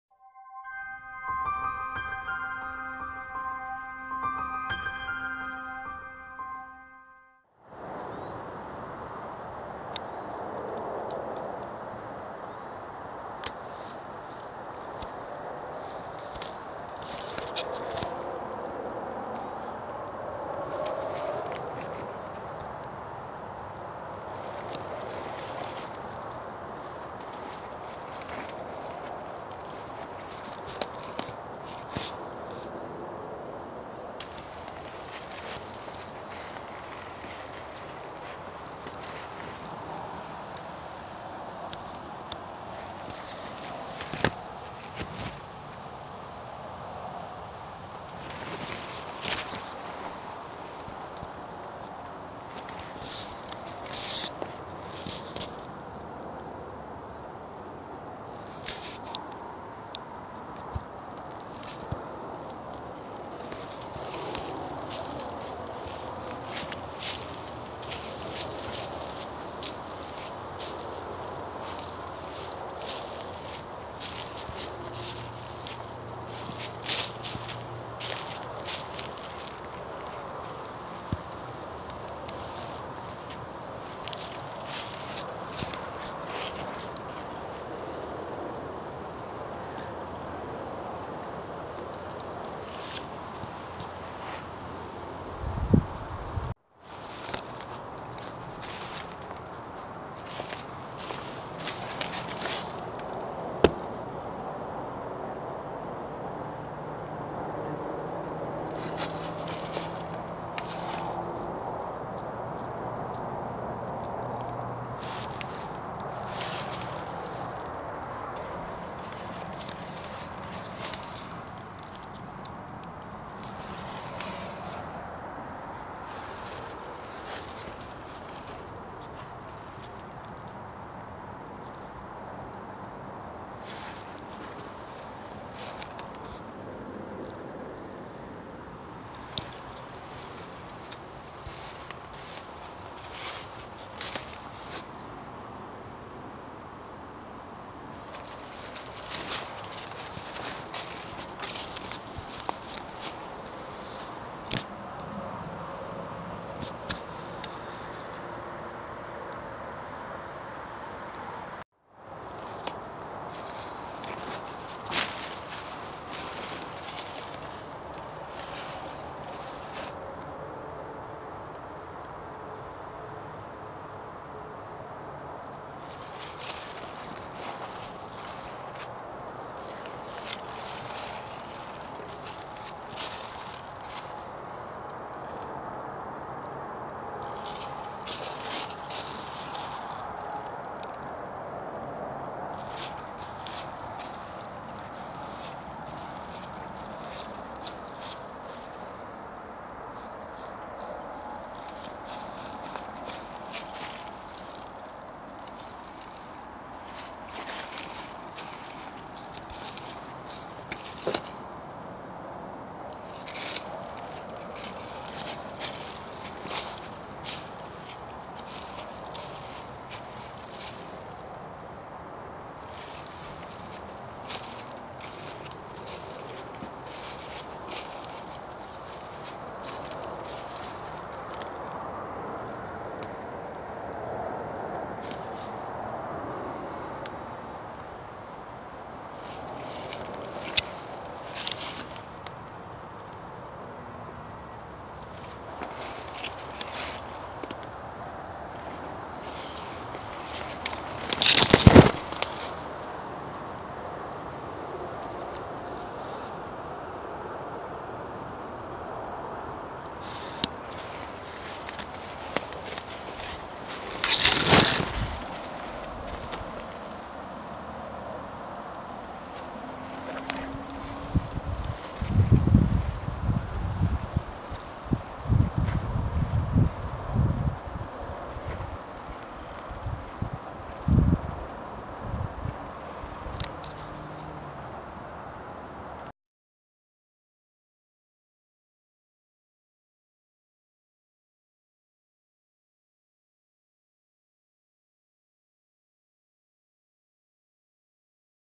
8 car graveyard in a forest - Urbex Finland
I found 8 abandoned cars in a forest - 5 Isuzu Troopers, Subaru BRAT, Volkswagen Beetle and Mitsubishi Lancer.